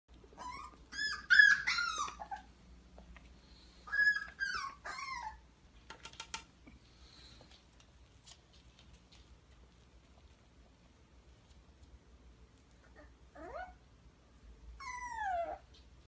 Diaper Puppy!
This puppy has his first diaper experience!